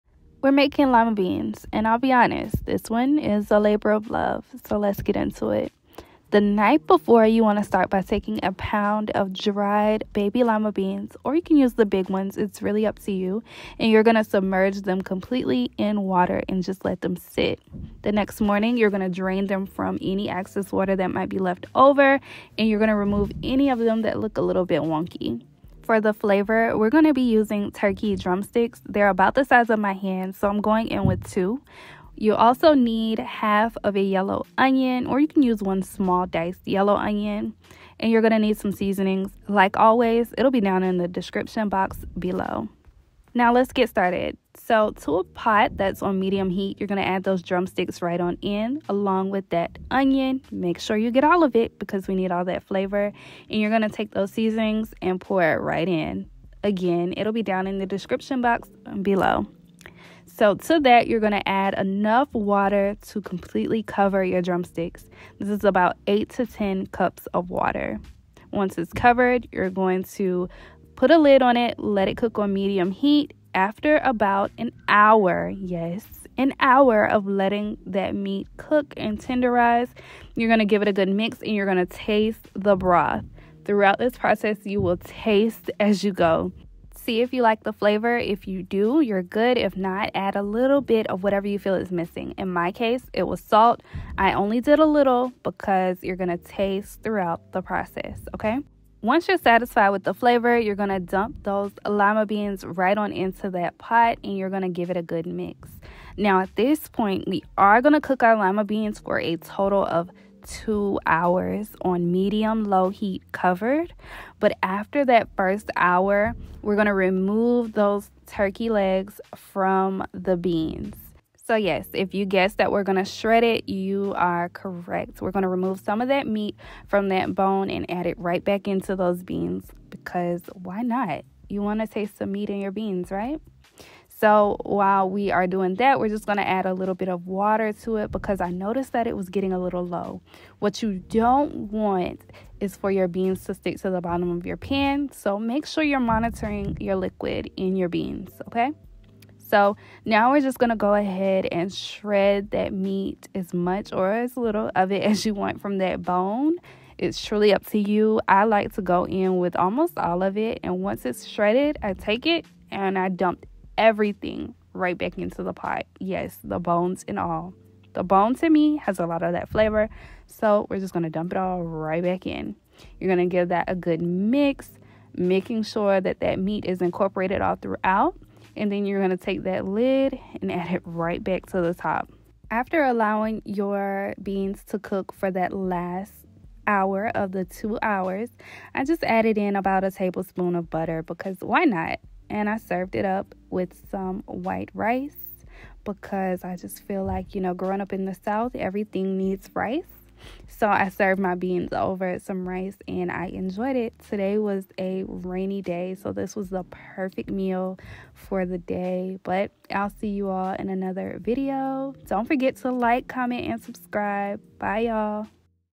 LIMA BEANS | Southern-Style Butter Beans
Hey y’all! In this video, we’re making LIMA BEANS or butter beans. Some people say it’s for people with a bit more age. I say it’s for people who love a good southern dish. 🤷🏽‍♀️

Here is the recipe:
1 lb. of DRIED Lima Beans
2 Smoked Turkey Legs (or a smoked meat of choice)
1/2 of a Yellow Onion, diced
8-10 Cups of Water
2 tablespoons of Chicken Bouillon Powder
1 tablespoon of Onion Powder
1 tablespoon of Garlic Powder
1 tablespoon of Mrs. Dash Original Blend
1 teaspoon of Paprika
1 teaspoon of Black Pepper

** NIGHT BEFORE **
Cover dried beans in water and allow them to soak.

** DAY OF **
Drain excess water and remove bad-looking beans.

1. Dump all ingredients except beans into a pot on medium heat. Add water until turkey drumsticks are covered.

2. After one hour, taste broth. Adjust seasonings as you’d like.

3. Add beans. Allow to cook for 1 HOUR with meat before removing and shredding meat. After the meat is shredded from the bone, return it to the pot.

4. Continue to cook beans on a medium-low heat for another hour. Taste. Serve over rice.